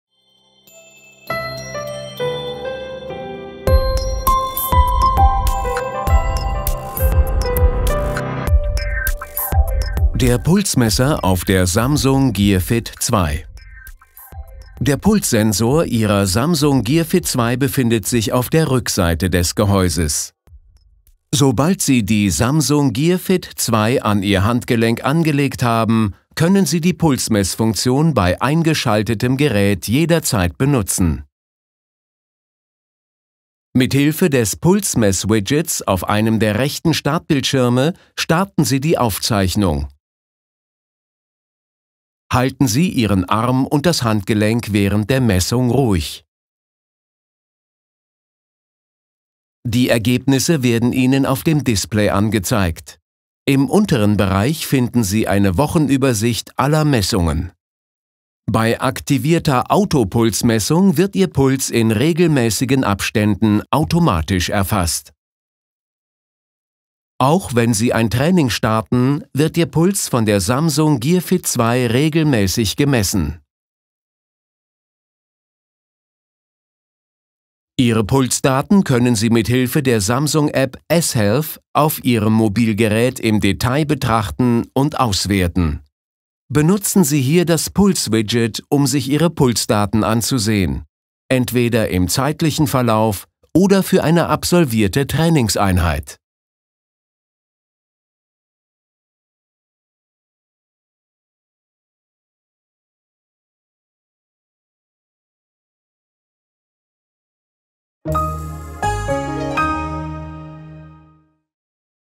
Samsung Gear Fit2: Pulsmesser
Die Samsung Gear Fit2 verfügt über einen integrierten Pulssensor, mit dem Sie Ihren Puls kinderleicht erfassen können. Wie Sie ihn benutzen und sich danach Ihre Pulsdaten auf der Samsung Gear Fit2 und in der Samsung App "S Health" anzeigen lassen, erklären wir in diesem Video.